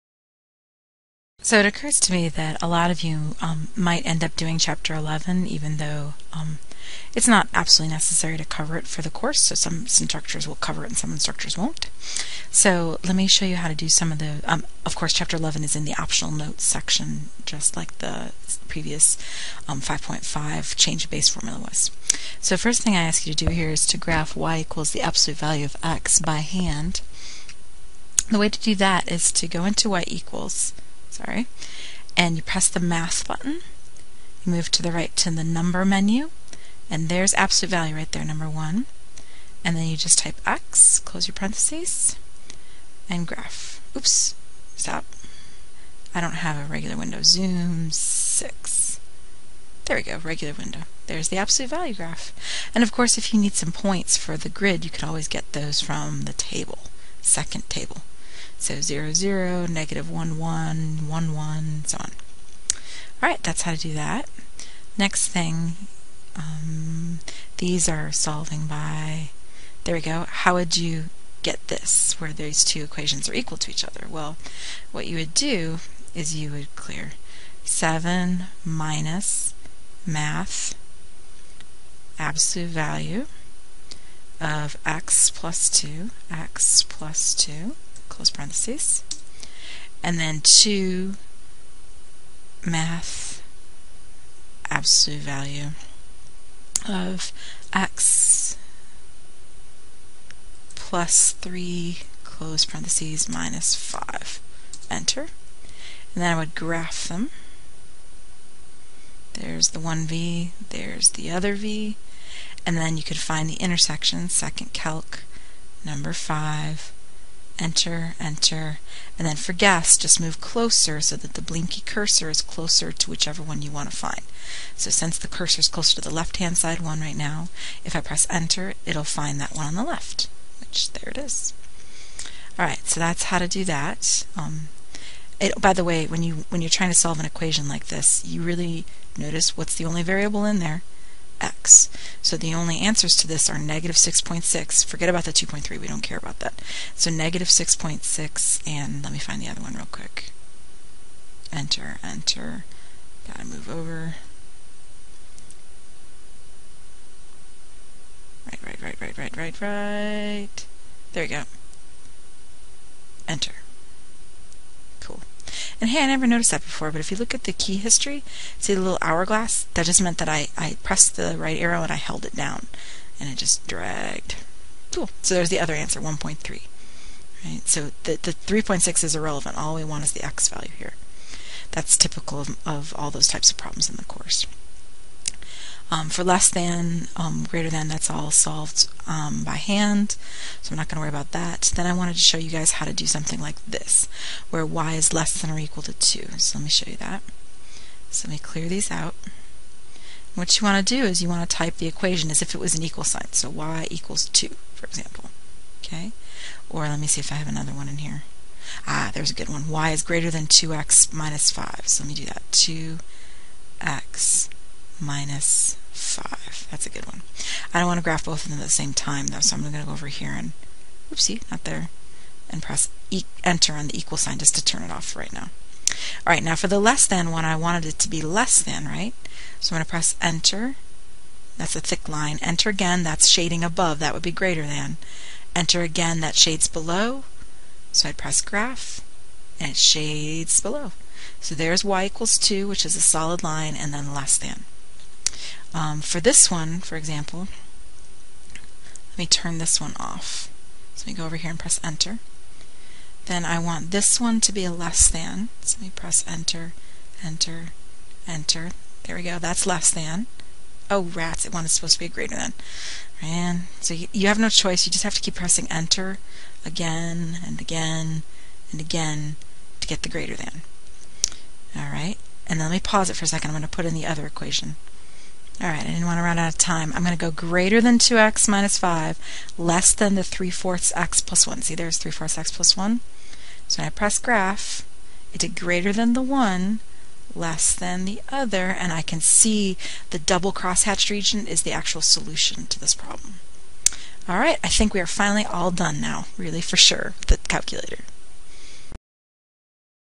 Ch 11 - Part 1 of 1 - Math 131 TI-83/84
Using a Ti-83/84 calculator in Math 131: Intermediate Algebra. This tutorial covers:

Graphing ABS functions
Using ABS to find the solution for a complicated equation
Graphing linear inequalities
Graphing a system of linear inequalities

Primarily meant for Math 131 students at Jackson Community College, MI. Textbook is Intermediate Algebra: Functions and Authentic Applications by Jay Lehmann, 4th edition.